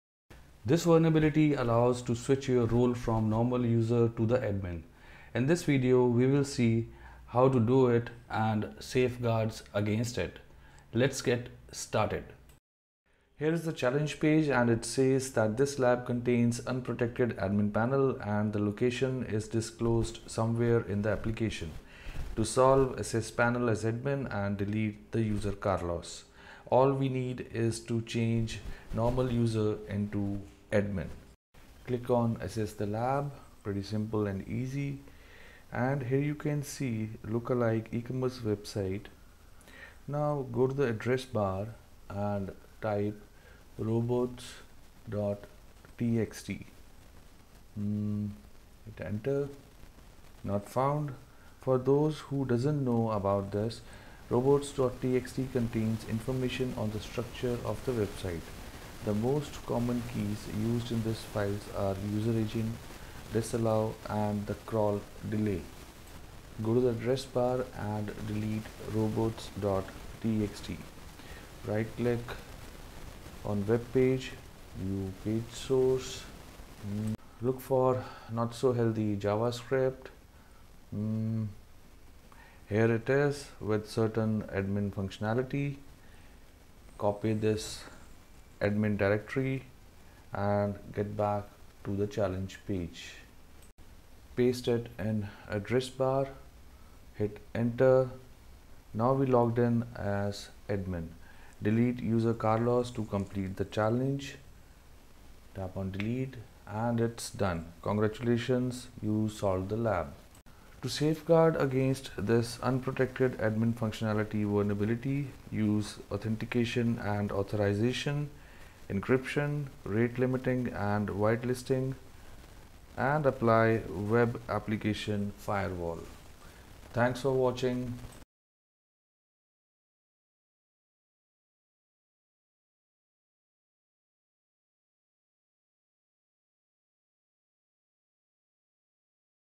From Normal User To Admin | unprotected admin solution
The web application vulnerability that allows bad actors to switch role and access the information they are not authorize to. In this video we will see solution for the challenge from website security academy and safeguards against it.